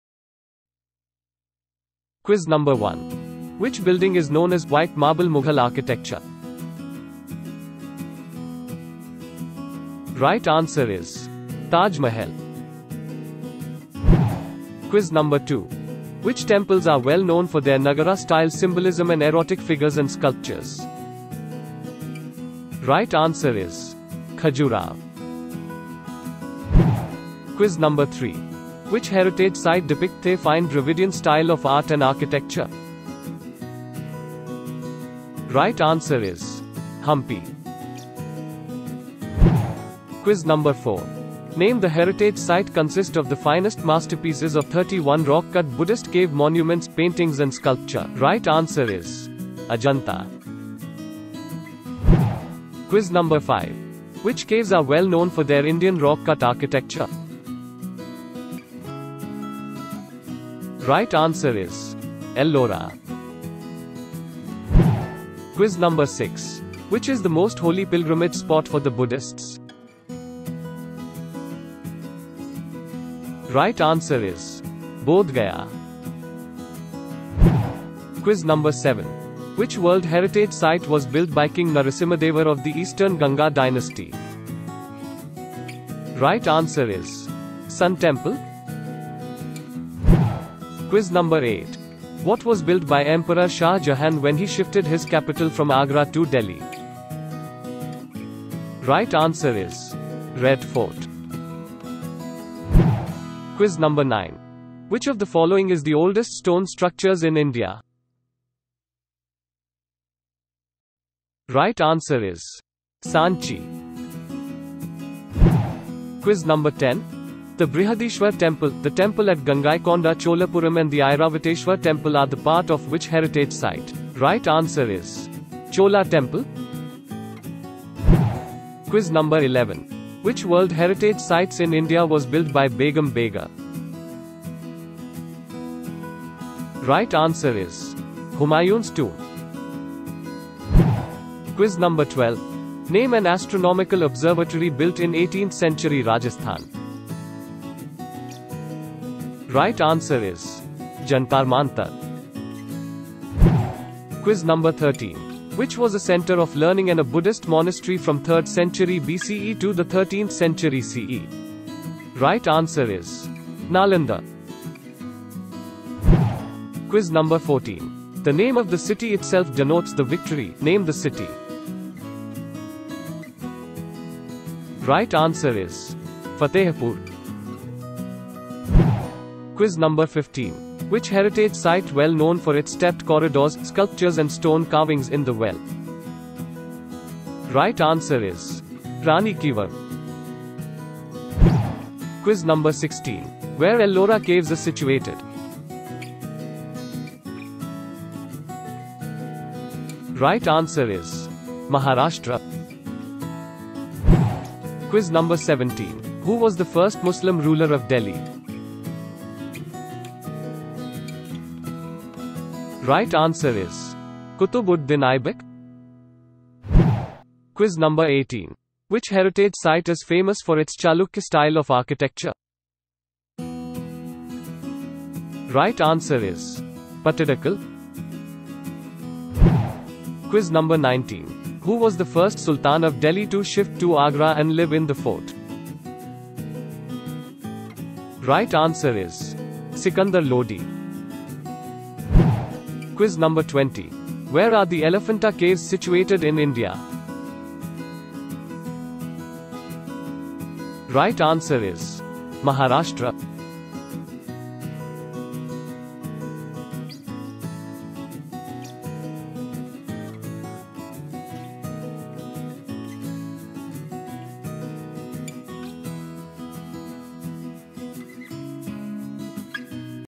Monument Quiz
Lets check out Indian Monument Quiz. You will find it full of information. Your comments are well appreciated.